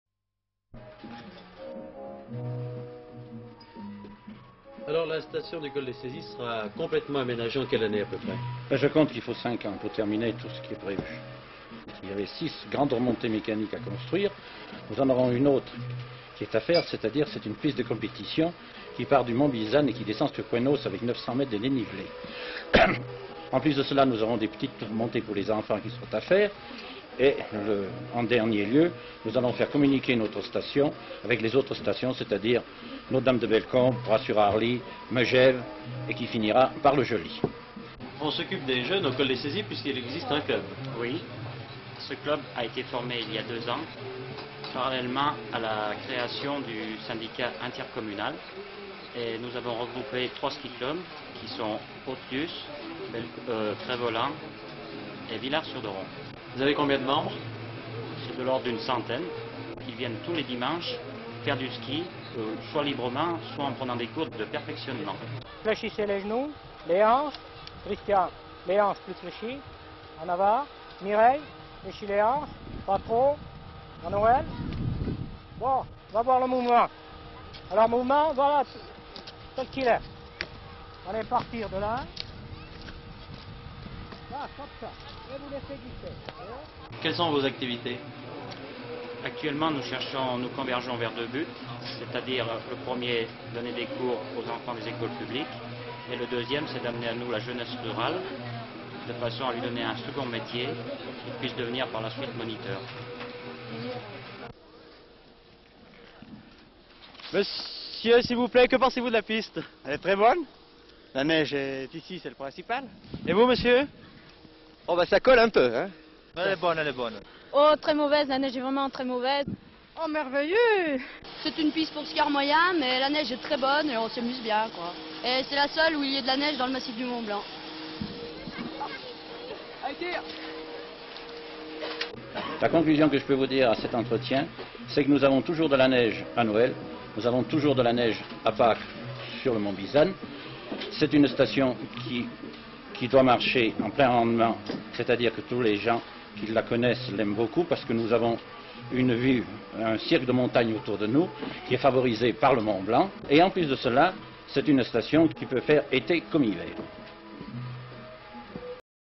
Col des saisies, 2ème partie
31 janvier 1964
 Présentation du projet de station de sports d'hiver aux Saisies, par des interviews, des images de montagne, de leçons de ski. Images d'archive INA